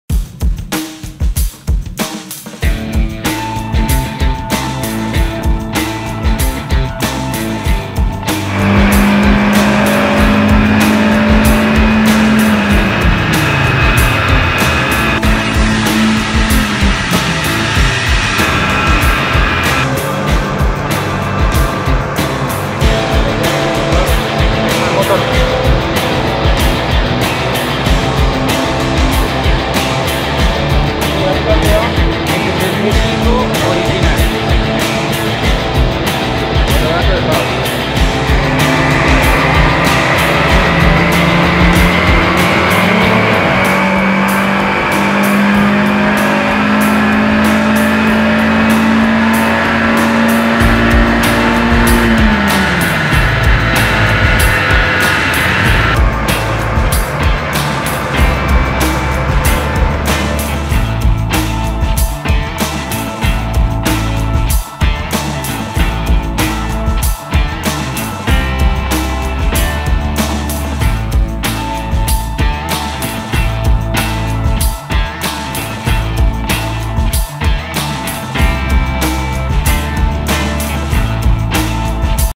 VW Gol G5 prueba en banco luego de repro en el curso de CHIP (ITAA)
Probamos VW Gol G5 original y luego una primera modificación para evaluar futuras mejoras en la repro